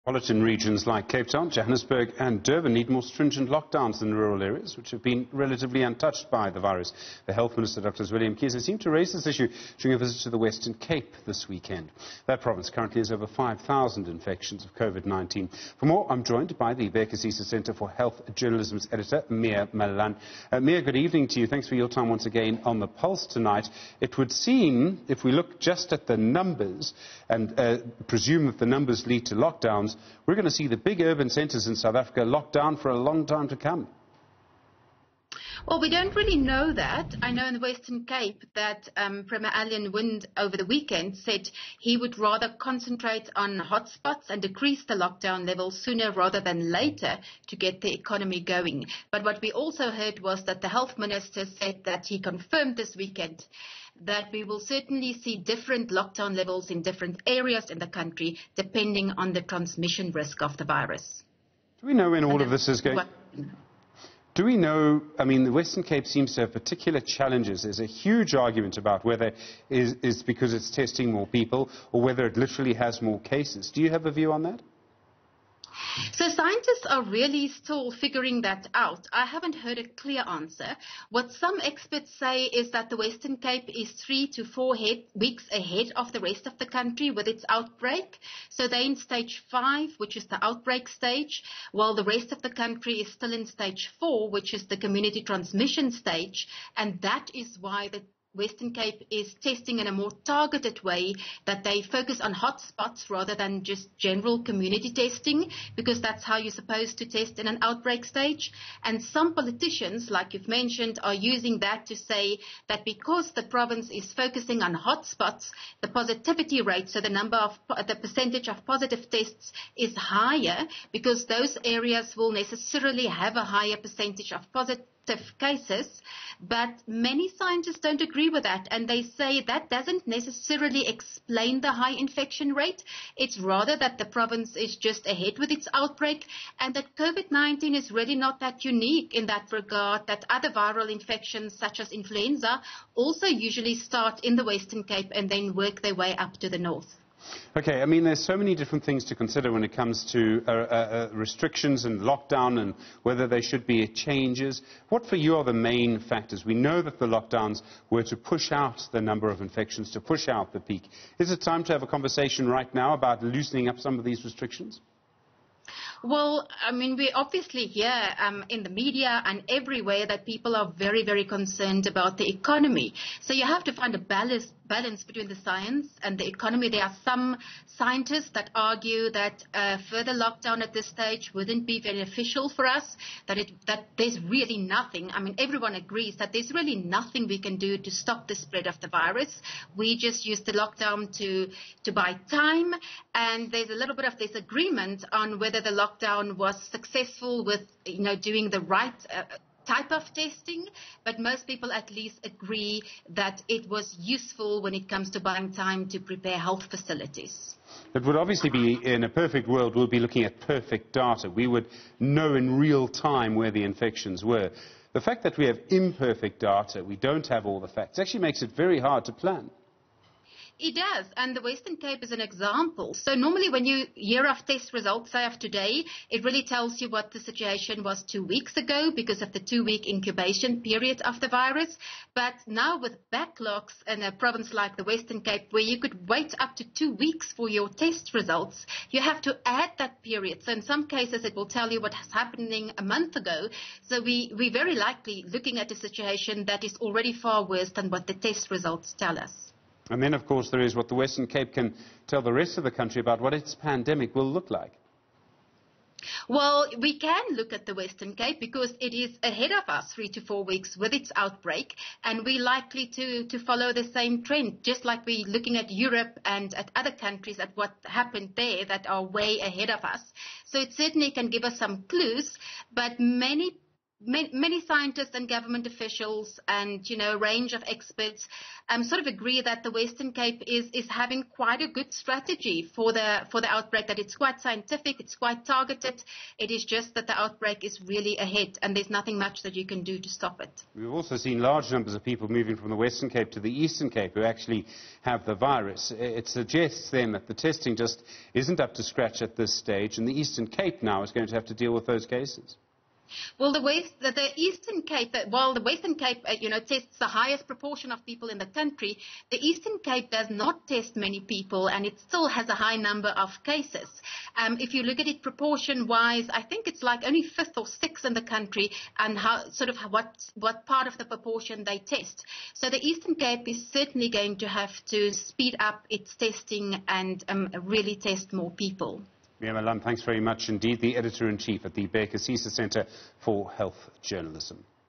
Stephen Grootes is joined by Mia Malan
The big cities have been mostly affected by COVID-19 compared to the rural areas. Mia Malan joins Stephen Grootes to further discuss this.